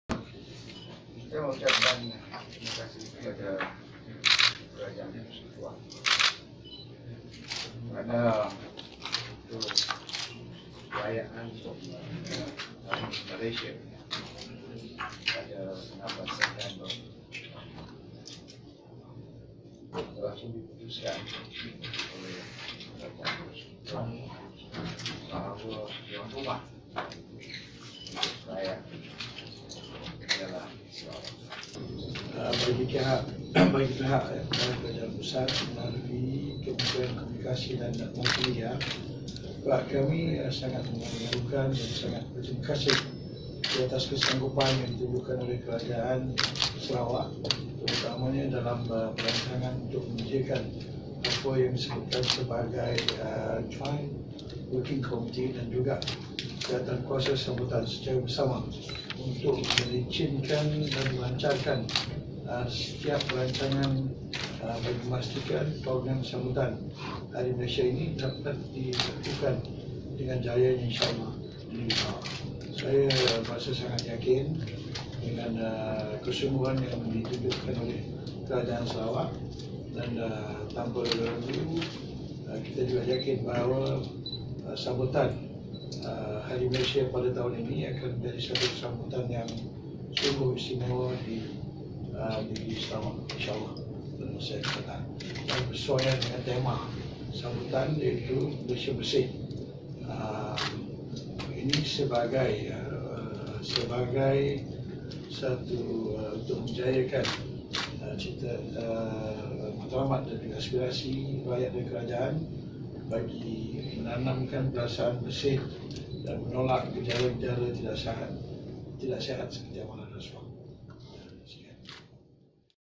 Sarawak tuan rumah sambutan Hari Malaysia 2019
17 Julai 2019

Sarawak akan menjadi tuan rumah Sambutan Hari Malaysia pada 16 September depan.

Sambutan kali ini bertemakan 'Sayangi Malaysiaku: Malaysia Bersih' .